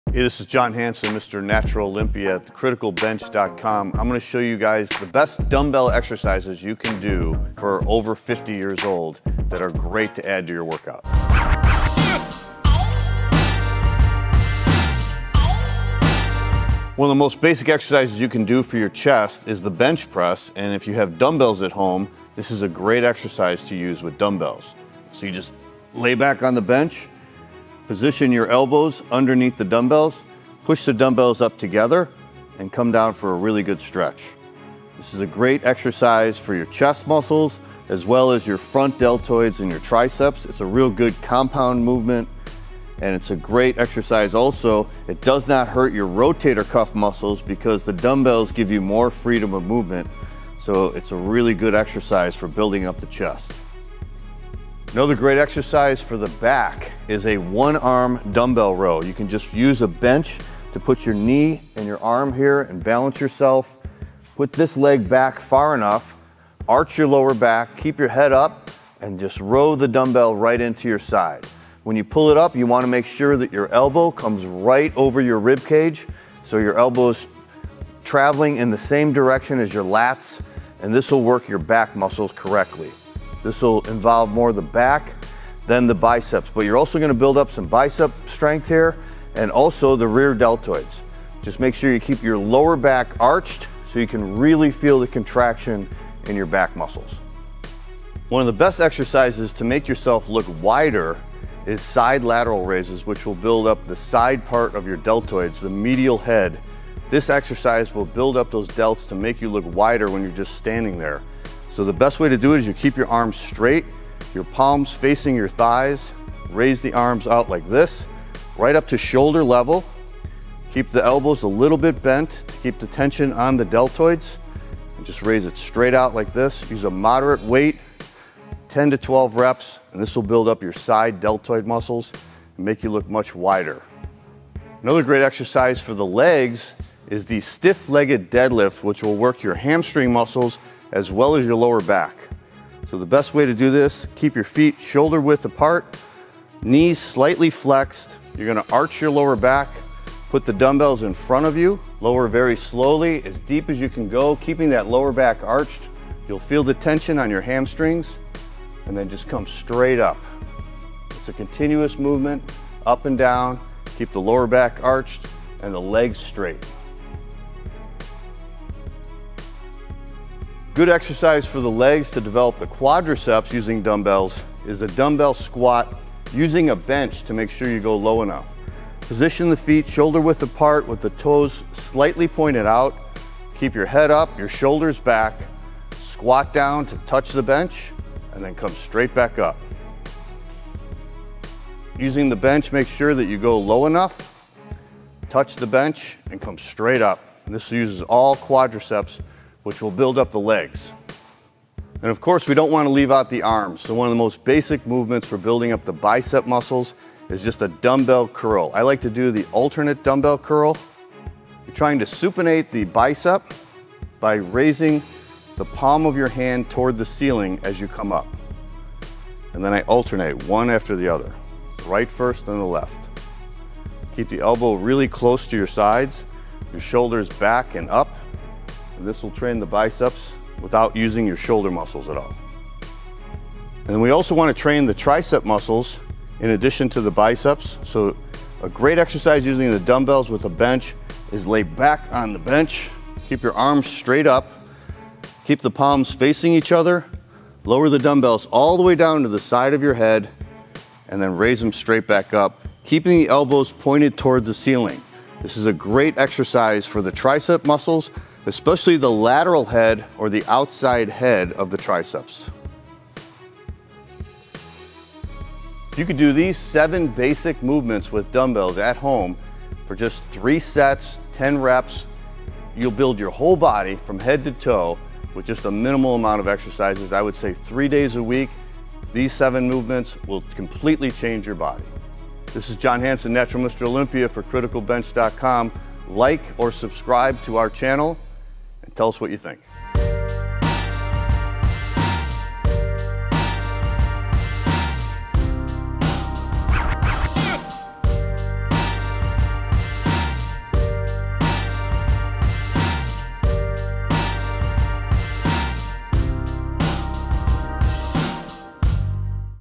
TOP 7 Dumbbell Exercises for Guys Over 50 (Time to Man Up!)
For guys over 50 years old, lifting weights and training hard is a must to stay in tip top shape. Legendary bodybuilder John Hansen shows you his Top 7 Dumbbell Exercises to do for muscle strength and size!

Natural champion John Hansen has been lifting weights for 40 years!  And at 55 years young, he know what exercises should be part of any man's lifting regimen.

With some dumbbells you can work your entire body effectively at any age.  John's TOP 7 exercises will hit every muscle from head to toe. If done consistently and with proper form, these moves will help to maintain muscle size and strength.

Lifting weights and resistance training is essential to all people for not just muscular strength but also for bone density and hormone production. Pushing your body physically with light to moderate weights becomes even more necessary as we age.

As people tend to become more and more sedentary with age, hitting the gym or designing a home workout with some dumbbells can be a real game changer to living a fuller, happier and more independent life.

Here are the TOP 7 Dumbbell Exercises for Guys Over 50

1. Flat Chest Press (bench press)
2. 1 Arm Row
3. Side Lateral Raises
4. Stiff Legged Deadlift
5. DB Squat (with bench)
6. Alternate DB Curl
7. DB Triceps Extension (elbow ext)

Doing this TOP 7 list of exercises will force your entire body to work and to grow.

Here's a great way to put this TOP 7 list together:

Do Exercise 1
Perform 2-3 Sets for that Exercise.
Rest 1 Minute between Sets.
Move to Exercise 2
REPEAT until you've done all 7 exercises!

If you're a beginner, be sure to use light weights and feel free to start with just 1 set for each exercise and take the rest you require.

As you get more advanced, you can do more sets, take shorter rest periods, increase the weight and so on. There are always variables to change to make the routine more demanding.

Hope you enjoyed this Dumbbell Training video with the Legendary John Hansen.